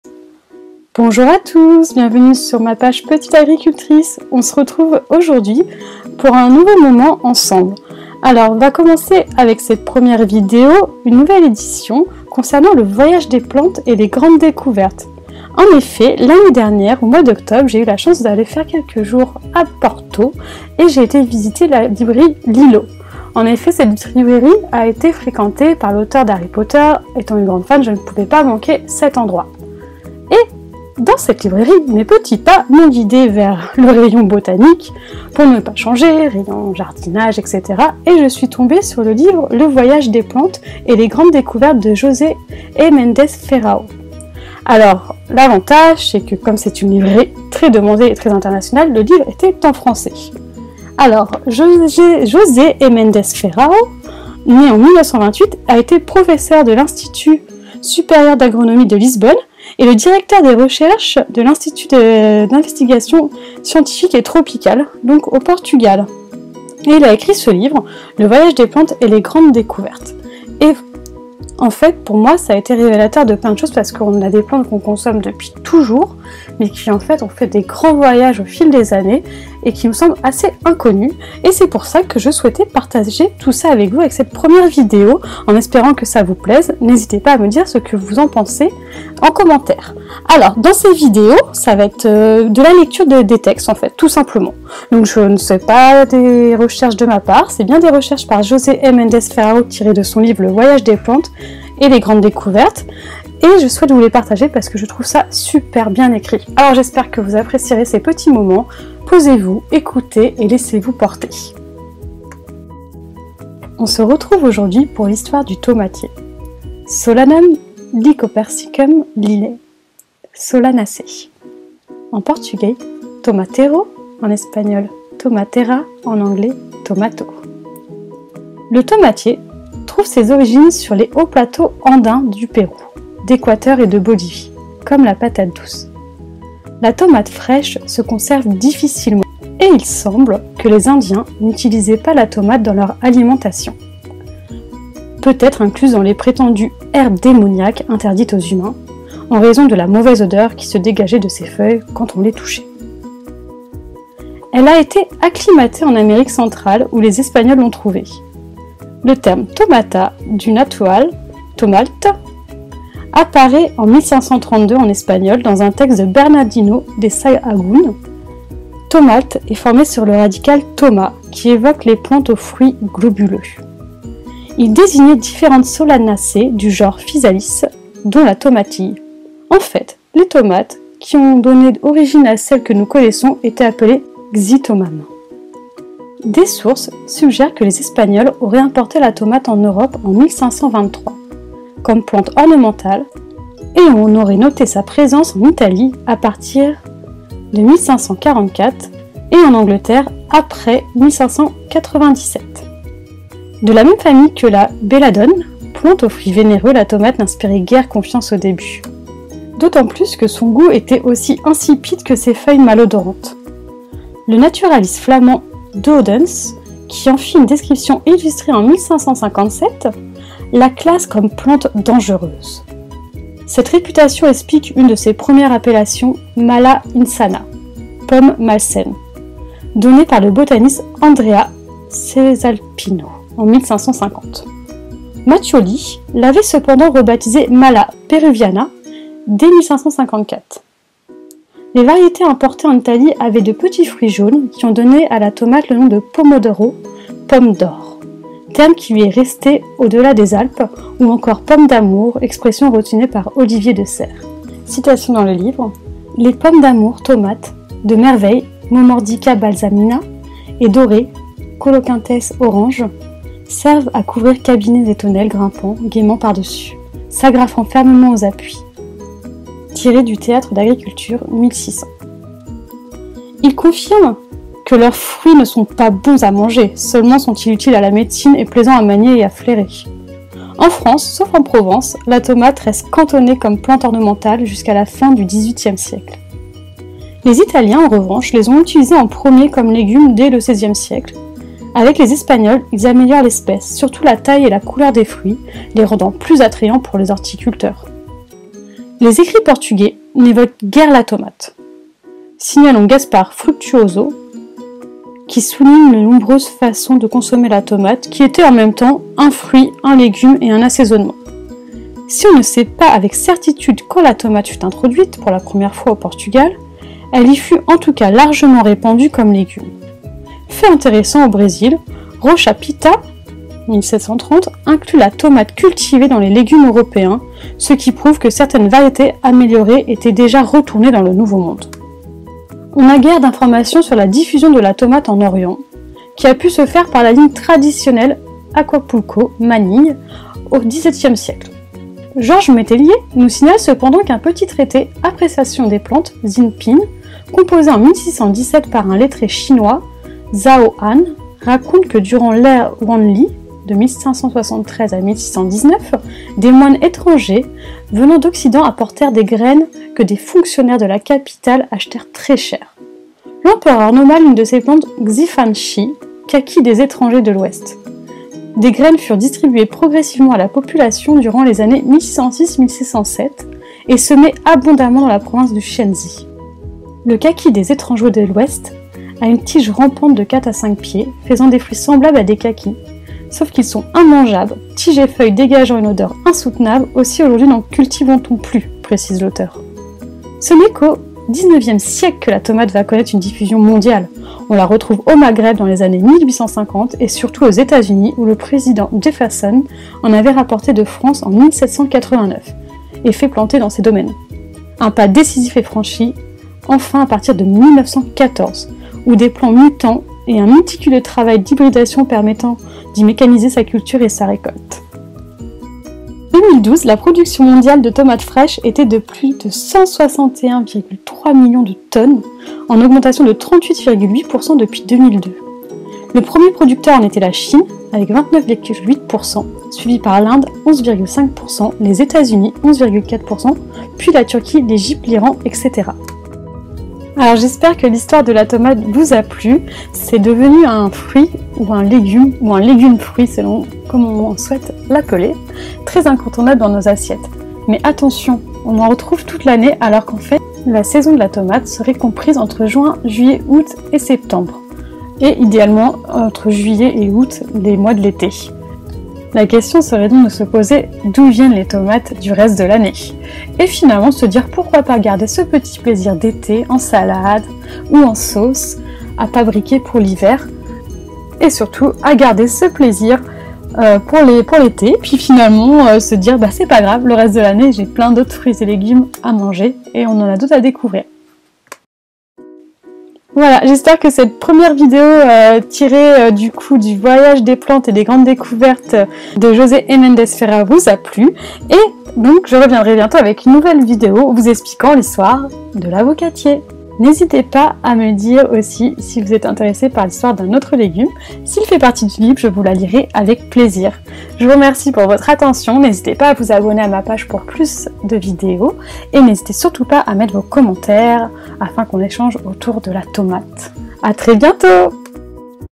L'HISTOIRE DE LA TOMATE // un jour, un légume (ou un fruit)
La Tomate avant d'arriver dans nos assiettes à fait de nombreux voyages.

Venez découvrir cela par les mots de José E. Mendes Ferrao dans son livre "Le Voyage des plantes & Les grandes découvertes", lus par ma voix sous forme de vidéo podcast.

A écouter à tout moment pour en apprendre plus sur la tomate, voici la première vidéo de ma série "Un jour, un légume (ou un fruit)".

Connaissez-vous des anecdotes sur la tomate? quels sont vos conseils pour la consommer?

Quel autre fruit ou légume souhaitez vous connaitre le voyage avant son arrivée dans notre assiette?